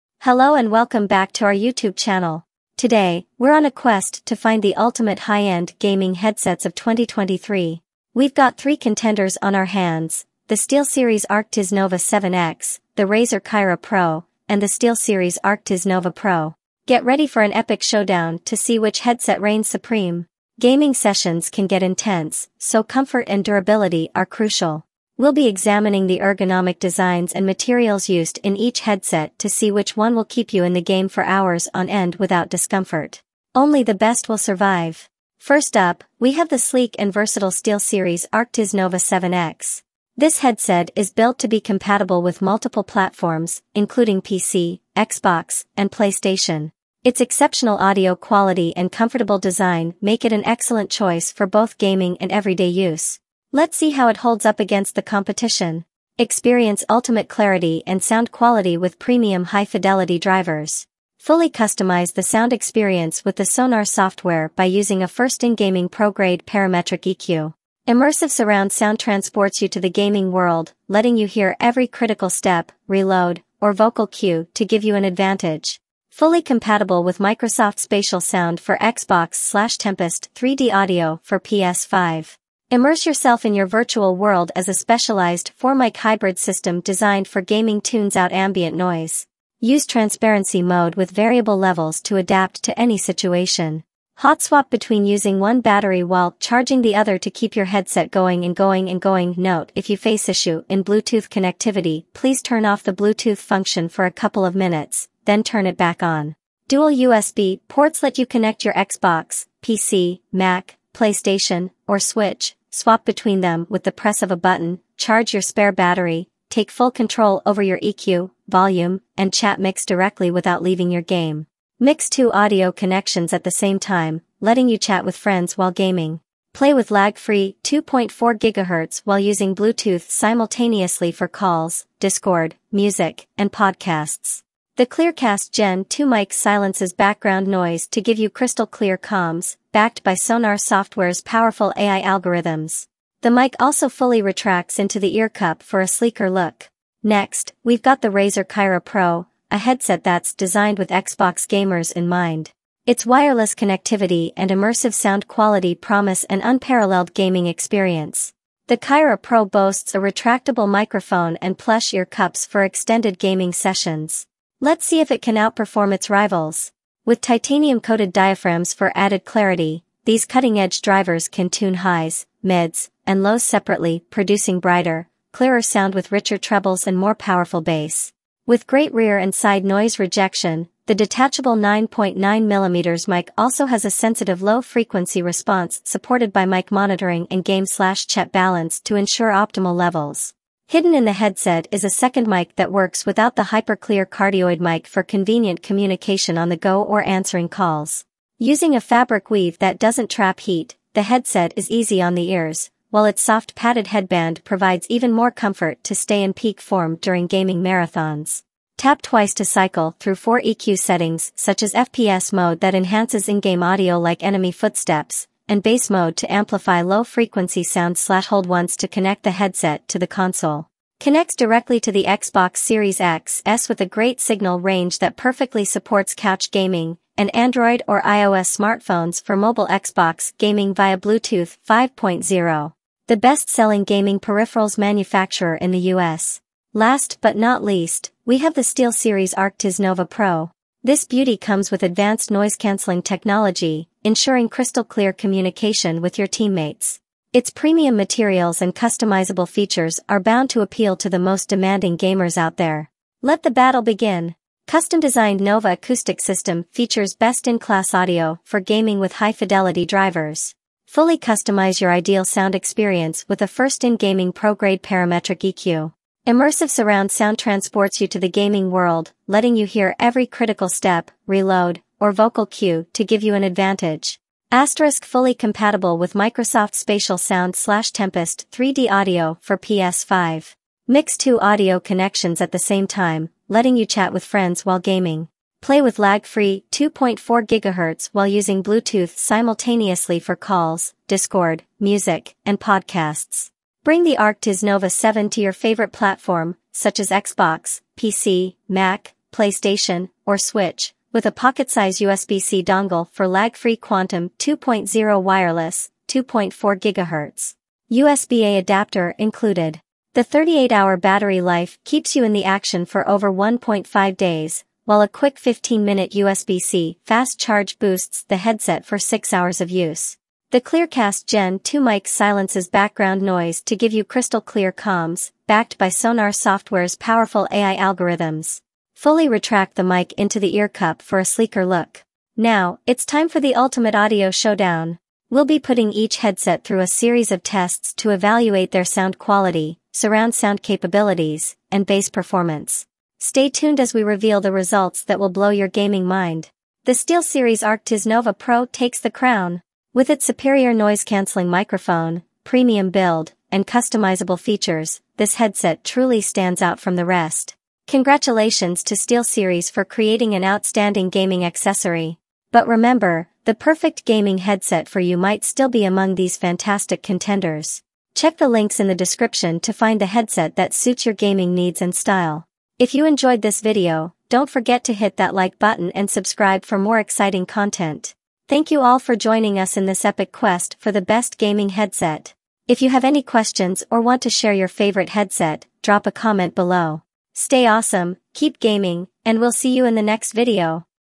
TOP 3 Best Headset for Xbox 2023 The Battle of Titans
In this epic showdown, we dive into the world of high-end gaming headsets to find the best contenders for 2023. Join us as we compare the SteelSeries Arctis Nova 7x, Razer Kaira Pro, and SteelSeries Arctis Nova Pro to determine the ultimate gaming headset for your gaming needs.

🎧 SteelSeries Arctis Nova 7x: Versatility and Performance
Experience unmatched compatibility with multiple gaming platforms, wireless freedom, and crystal-clear audio quality. The Arctis Nova 7x delivers an exceptional gaming experience.

🎧 Razer Kaira Pro: The Xbox Gamer's Delight
Tailor-made for Xbox gamers, the Kaira Pro offers seamless wireless connectivity, immersive THX Spatial Audio, and a retractable microphone for clear communication.

🎧 SteelSeries Arctis Nova Pro: Uncompromising Performance and Customization
Designed for serious gamers, the Arctis Nova Pro boasts advanced noise-canceling technology, premium build quality, and personalized sound settings.

Which headset will be crowned the winner? Watch the video to find out!

🔗 Links to the featured headsets:
►SteelSeries Arctis Nova 7x - Best Xbox Headset for Most People
★US Prices
★UK Prices

►Razer Kaira Pro - Best Xbox Headset For The Money
★US Prices
★UK Prices

►SteelSeries Arctis Nova Pro - Best Overall Xbox Headset
★US Prices
★UK Prices